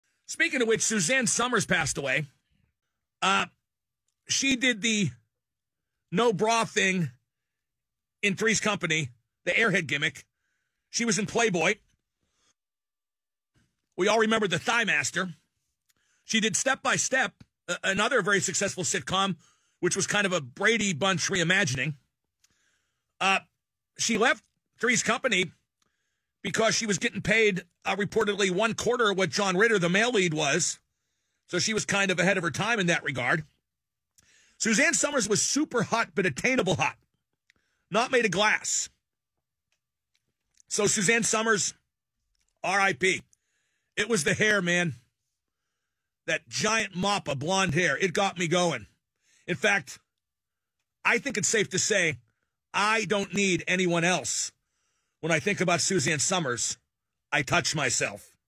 Suzanne Somers RIP
MM pays tribute in multiple ways to Suzanne Somers.